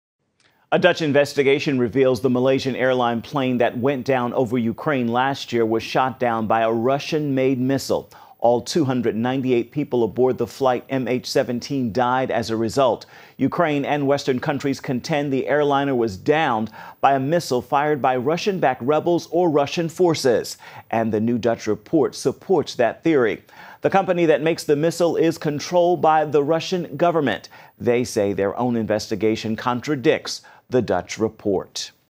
Report: Malaysia Flight Downed by Russian Missile
The Malaysian Airlines plane that blew up over Ukraine last year was shot down by a Russian-made Buk missile, a Dutch investigation revealed Tuesday.